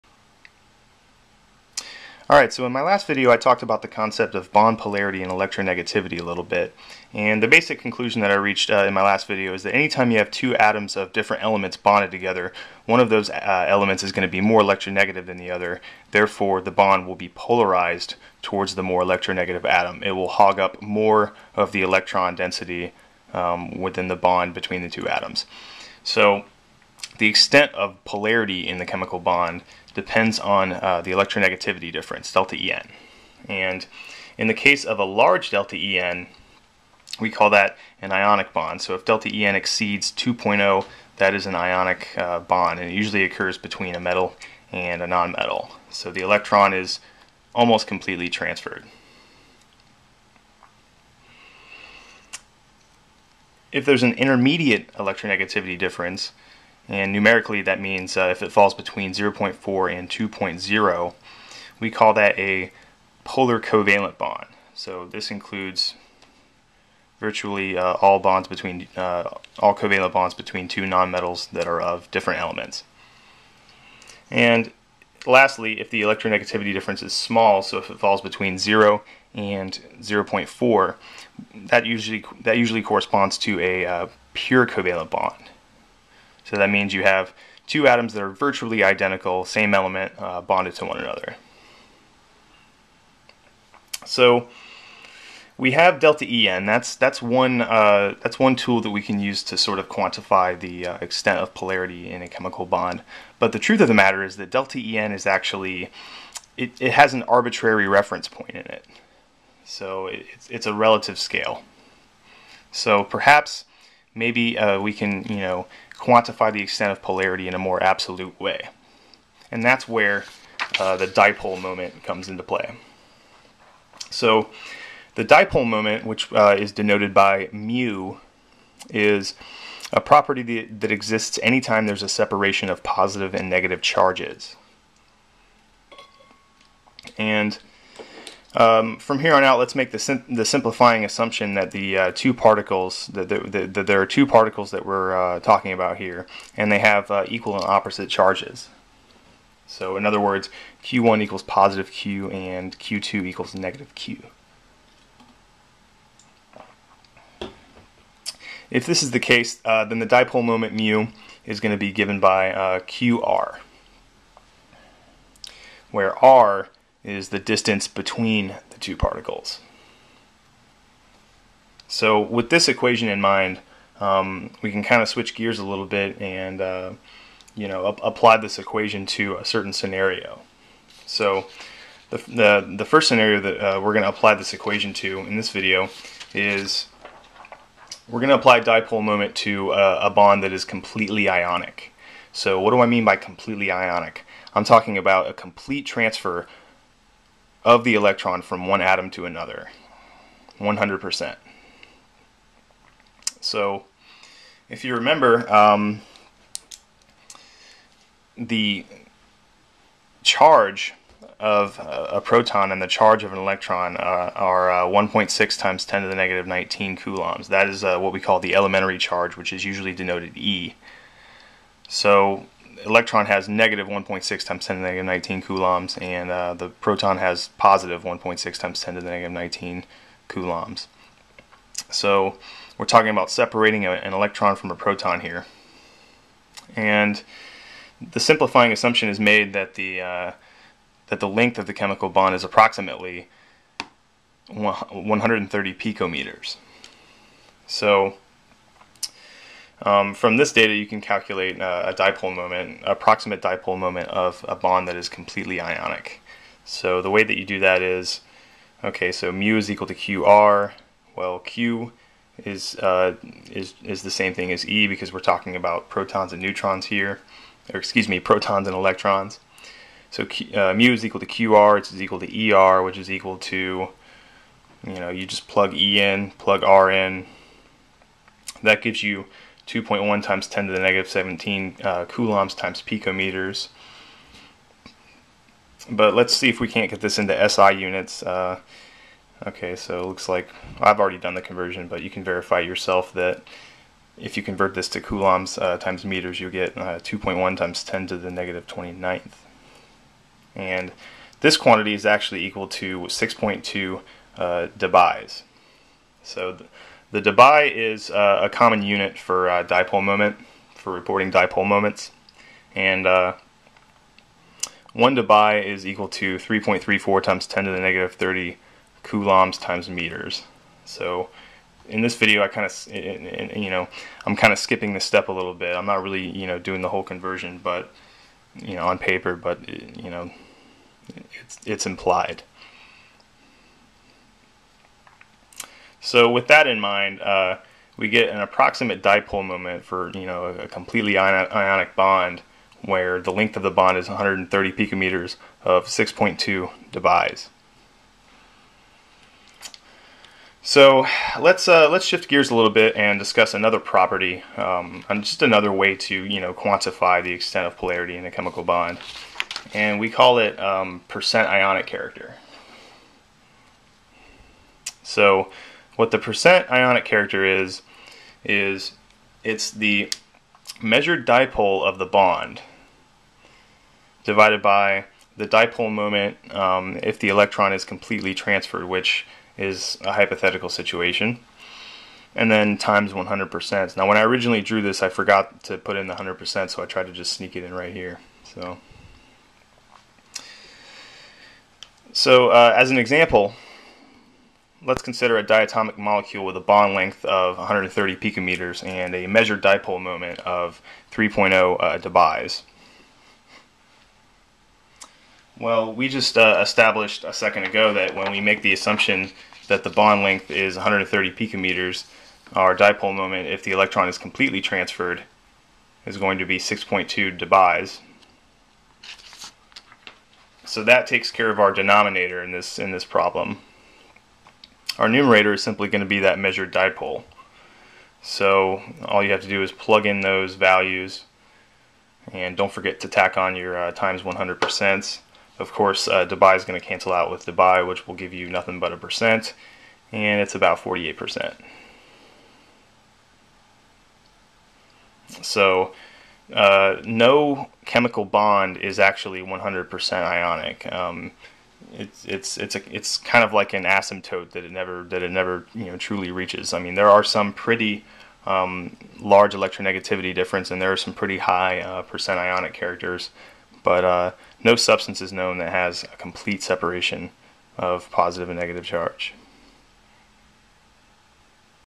Lewis Theory V: Quantifying Polarity of Chemical Bonds - Dipole Moment and Percent Ionic Character
This video goes further in depth on the subject of bond polarity.  While electronegativity differences are good for comparing relative polarities of different types of bonds (e.g. C-O vs C-N), the electronegativity scale is relative, not absolute.  Dipole moment and percent ionic character are two absolute measures of bond polarity.